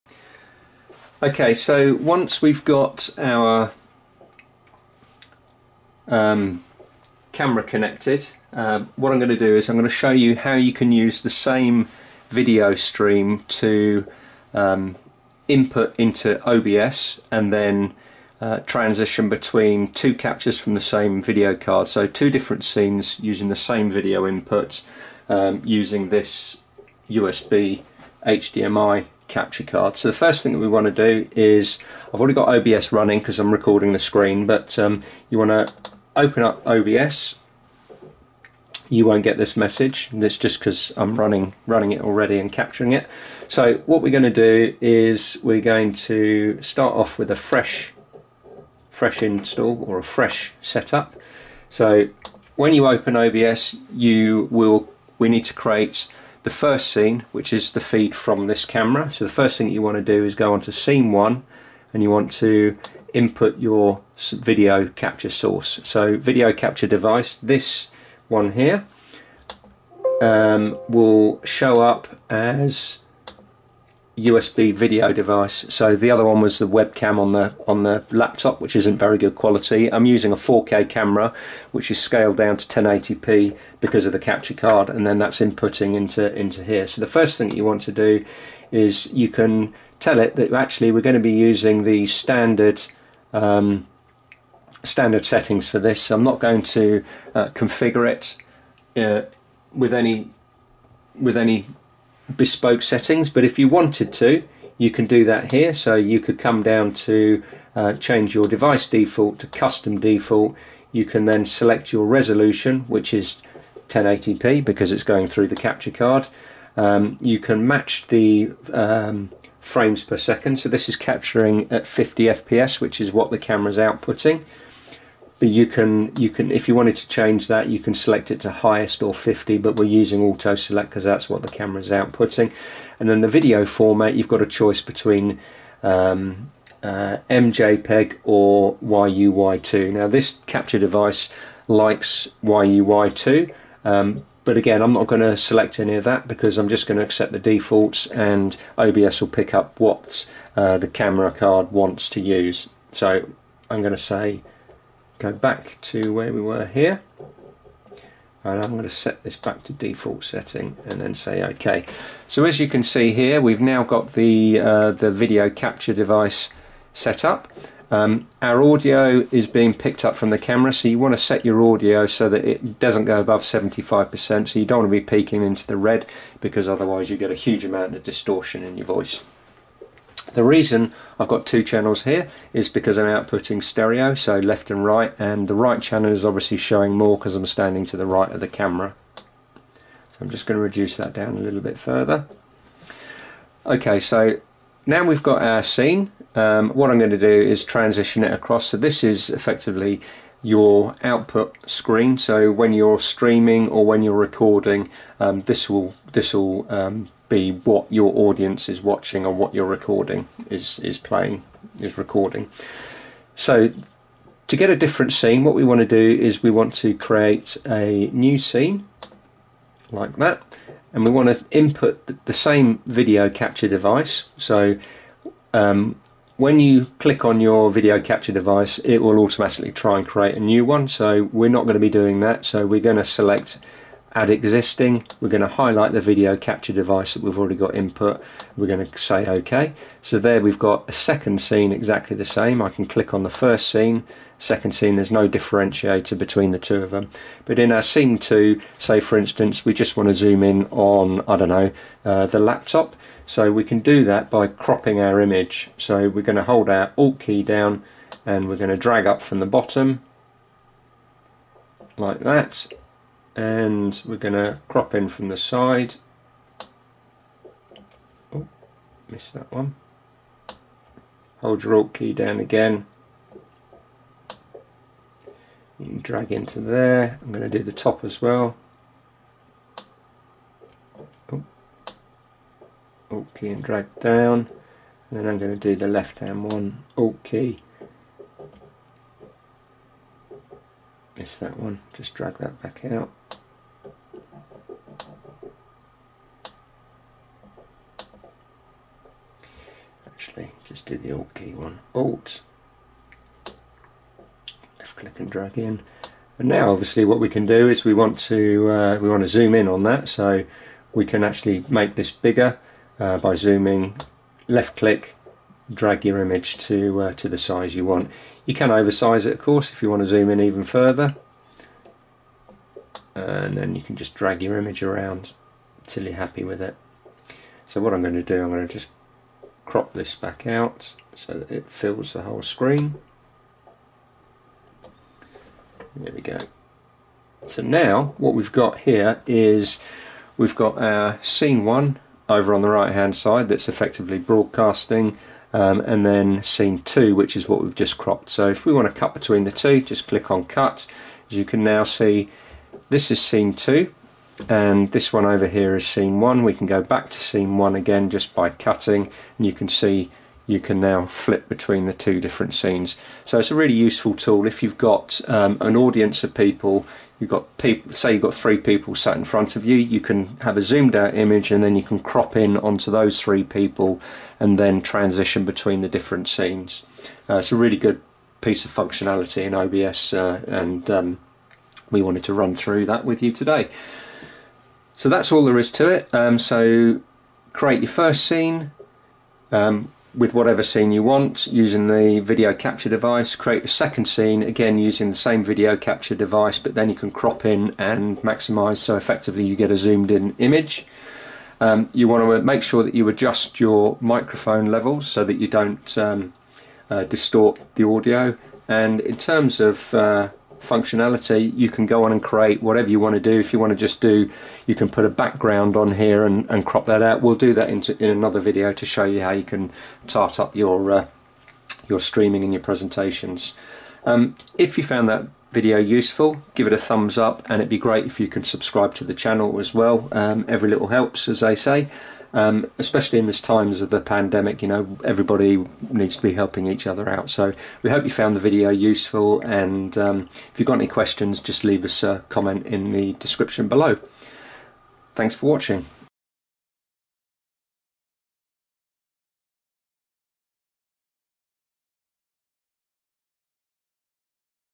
OBS - Create multiple scenes from a single HD camera source
If you only have a single HD or 4K camera and want to create multiple views from it for broadcasting then this video is for you. We should you how to create multiple scenes/views from a single HD video capture source in OBS.

In this video we are using a low cost Amazon purchased HDMI to USB3.0 capture card to grab video from our Sony FDR-AX53 video camera. This allows us to create high quality different scenes from the same camera.